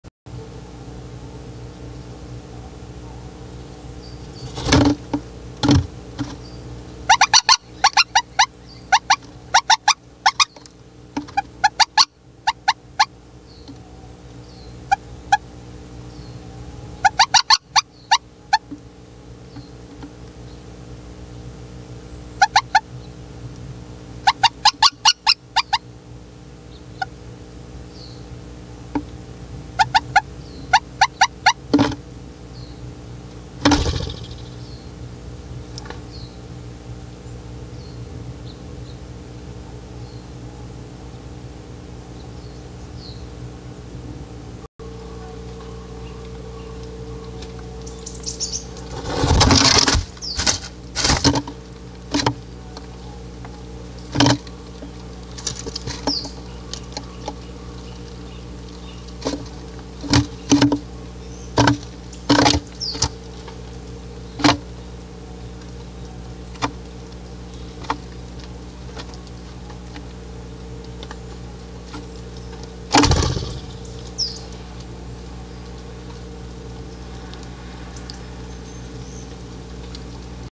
フクロウ巣箱を訪れたアオゲラ（変わった鳴声Ver.）2020年2月12日
はなの花菜の

​９：３４：１５よくなく鳥さんがきていました。たぶん、アオゲラかアカゲラだと思いましたが、ずっと鳴いていたのがめずらしくて一応ログに時間をのこします。

１２：０６：４６アオゲラさんが巣箱にはいりました。急に入ってきた様子がおもしろかったです。